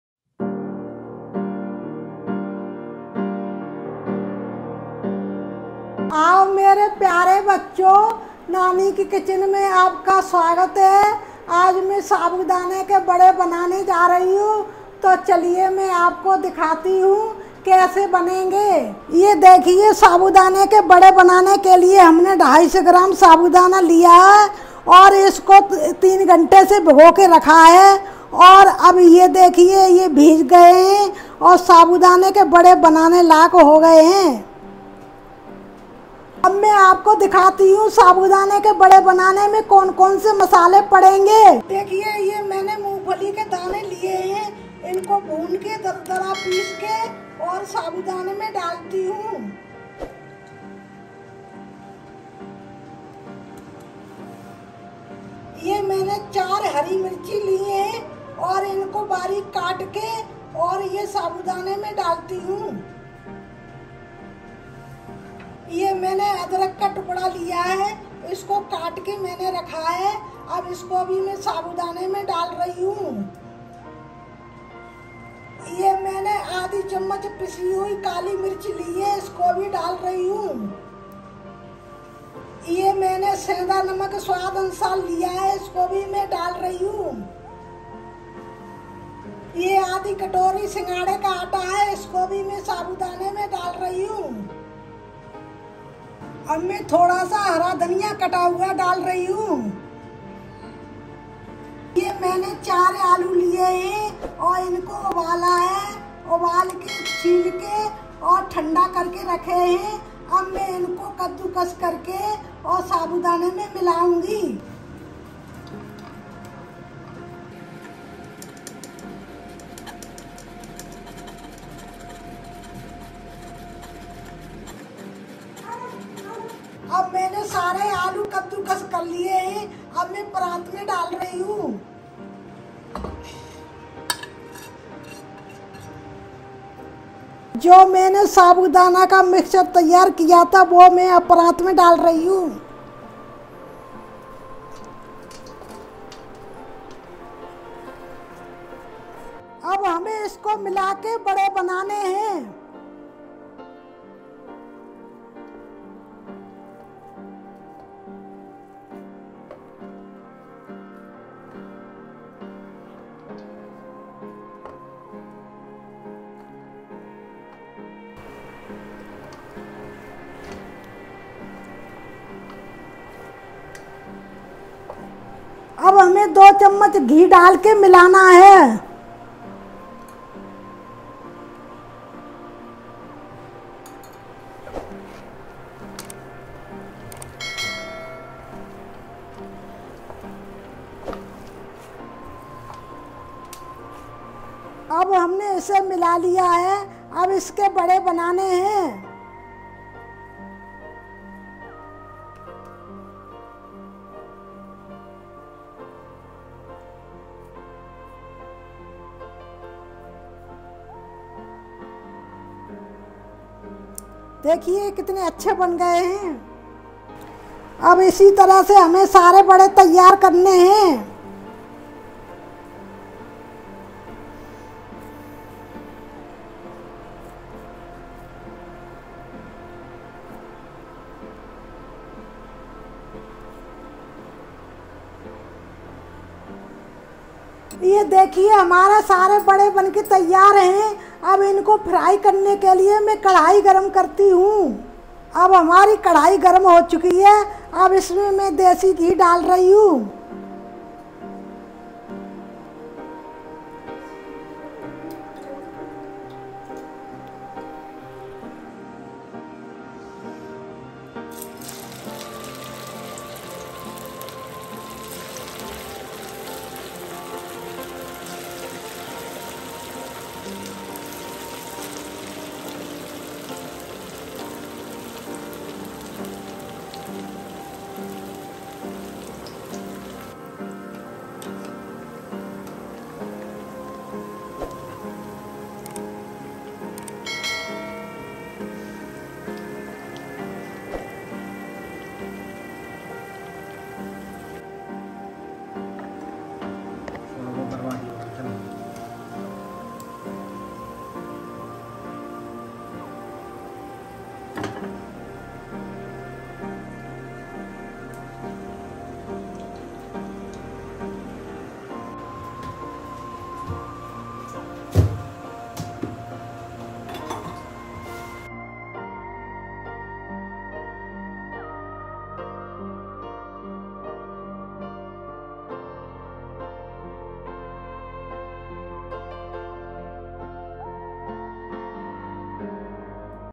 Kurkure Sabudana Vada Banane ki Recipe | कुरकुरा साबूदाना वडा | Meri Nani Ka Kitchen hindi recipe
Here is a Tasty and delicious Crispy Sabudana Vada recipe made by nani , i hope you guys will like this vedio and support this channel with lots of like and comments thank you.